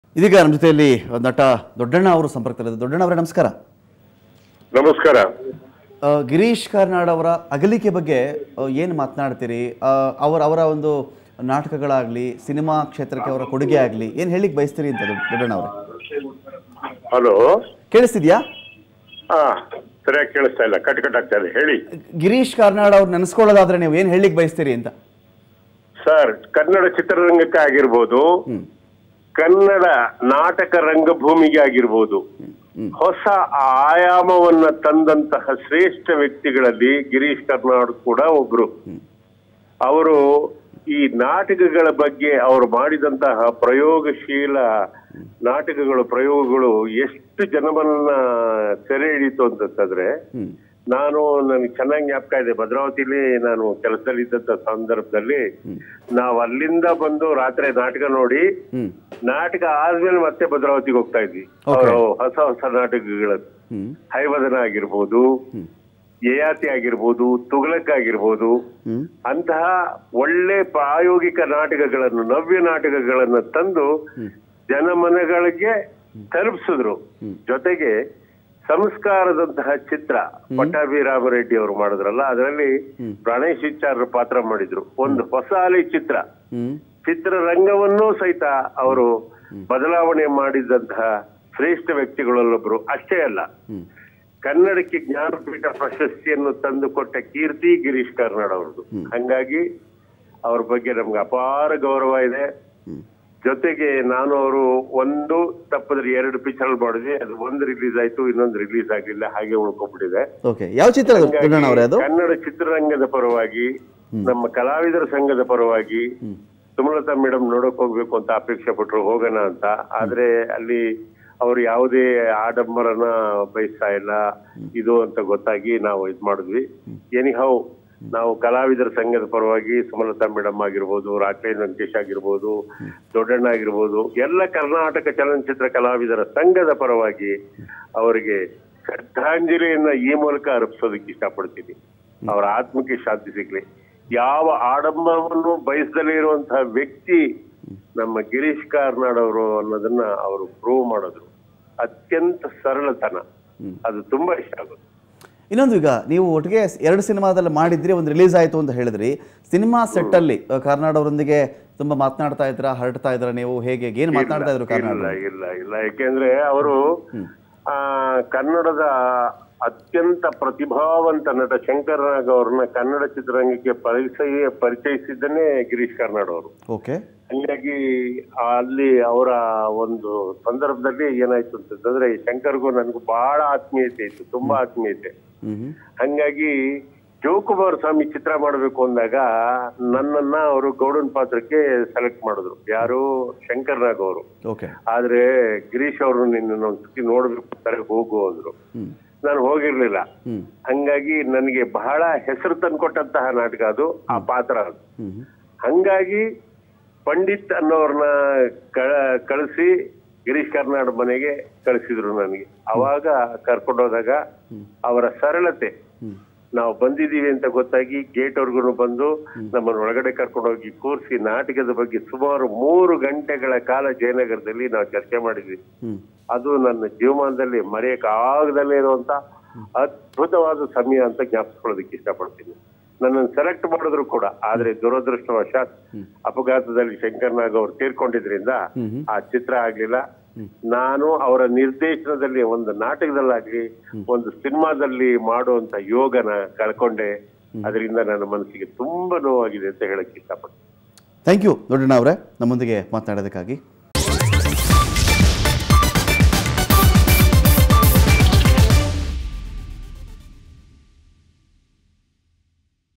Actor Doddanna Shares His Acting Experience With Girish Karnad & Express Condolence
Doddanna Reacts To Suvarna News